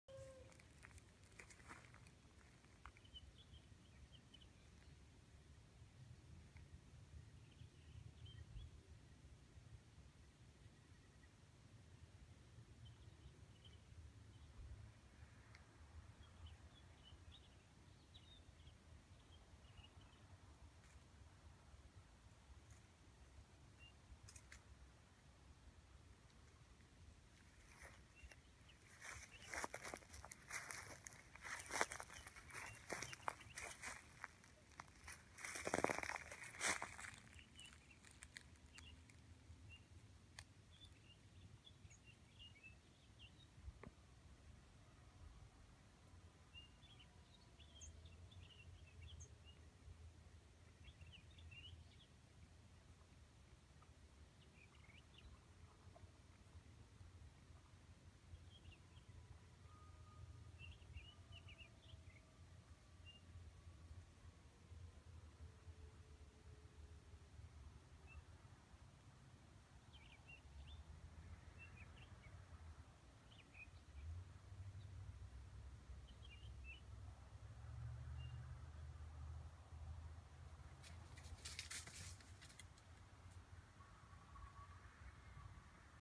Enjoying deer with soul haunting coyote howling
I  was enjoying a deer filled morning when I heard the soul haunting coyote howl.  There were two bands communicating.. right at the beginning you can hear the ones close to me... the pack got quiet but they are still present in the background!